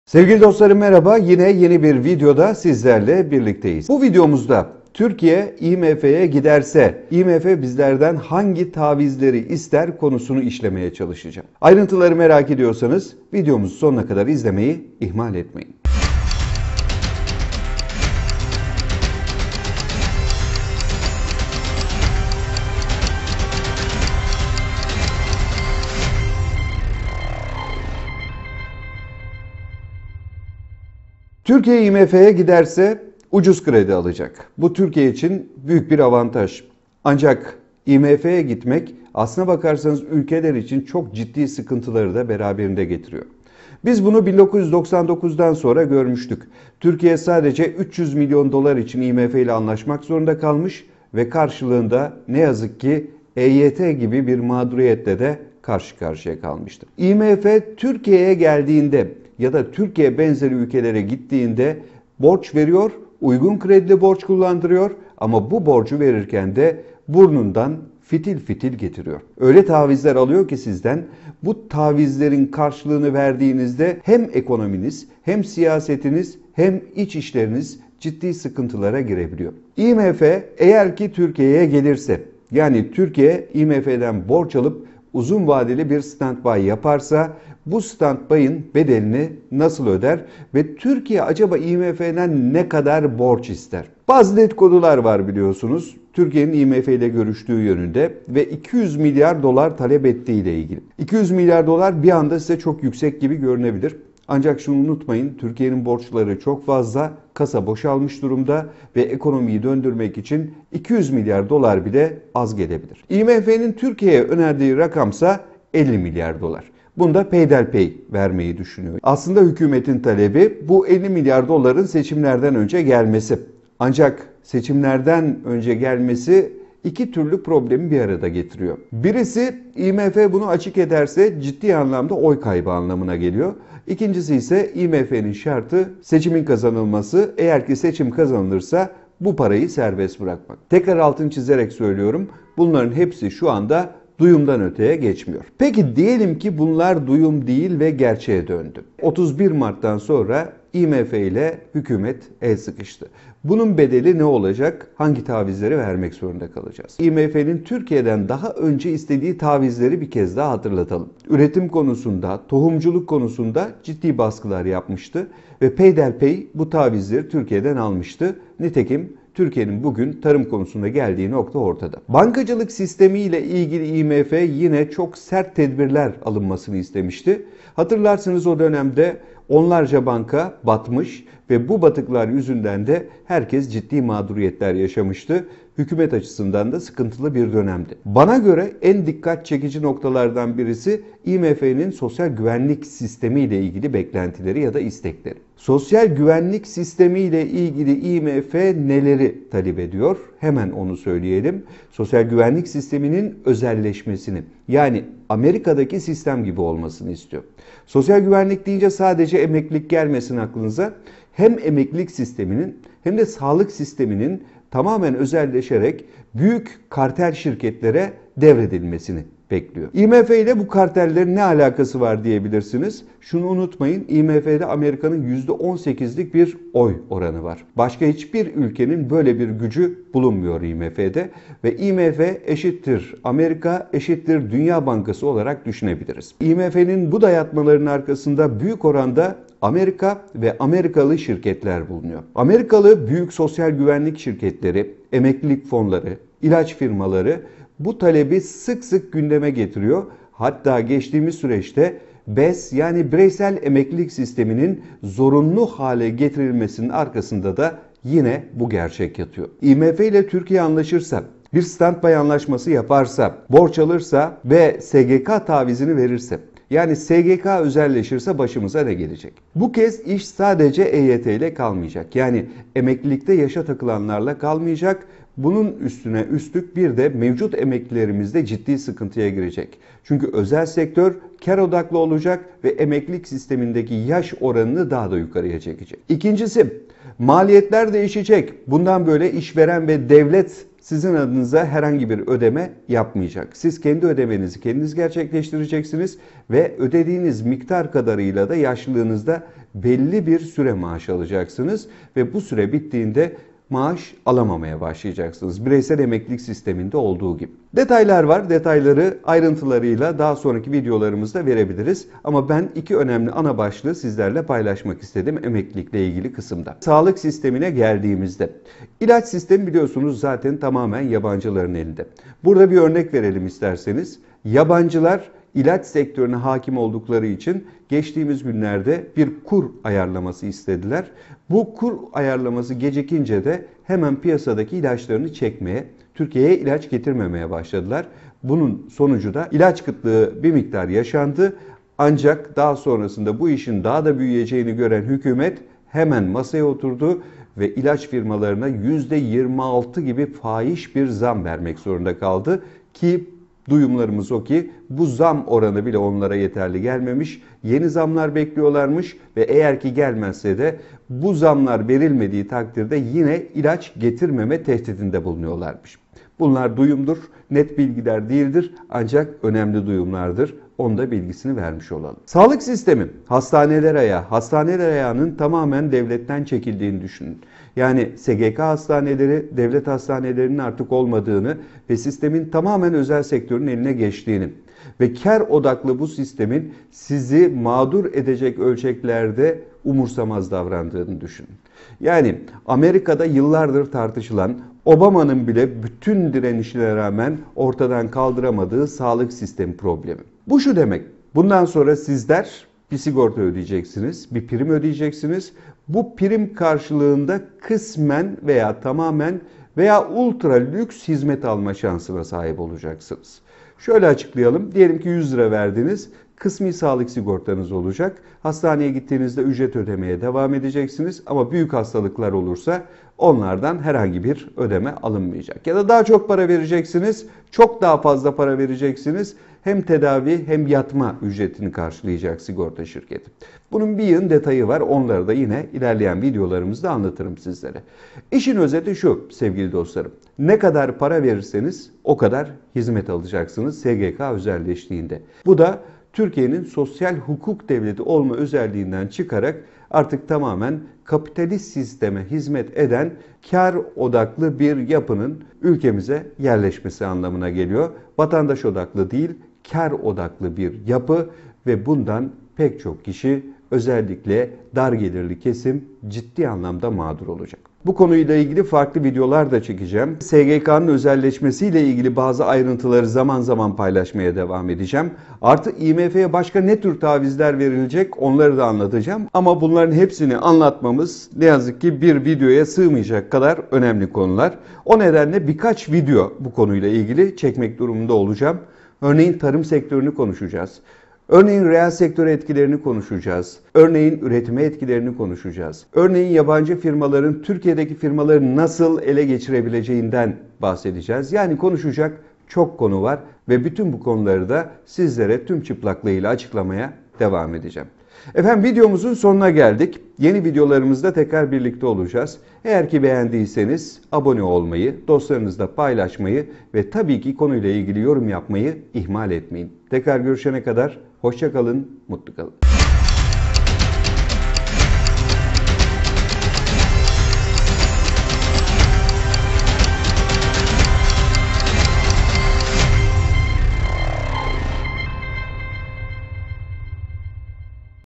Ekonomik Kriz Çıkar IMF Gelirse Emekli Olmayı Unutun!!!
Ekonomi Gündeminin en önemli sorularından birisi hükümet yakın zamanda IMF ile masaya otorabilir mi? ve eğer IMF ile Türkiye arasında bir Stand By anlaşması yapılırsa Türkiye IMF ye hangi tavizleri vermek zorunda kalır. Türkiye ABD arasında yaşanan ilişkiler ve Trump yönetimi IMF yönetimine Türkiyeden çok daha fazla taviz alması için baskı yapar mı? IMF özellikle 2000 yılından önce Türkiye hükümetinden istediği SGK Sosyal Güvenlik Sisteminin özelleştirilmesi konusunu tekrar gündeme getirir mi? ve tüm bu süreçlerin bizlere yani Türkiye Cumhuriyeti vatandaşlarına etkisi ne olur? Bu ve buna benzer pek çok sorunun cevabını IMF Gelirse Verilecek Tavizler videolarımızda anlatmaya çalışacağız. Bu bölümde SGK özelleşmesi ve IMF konusunu ele alıyoruz. İyi Seyirler... Selçuk Geçer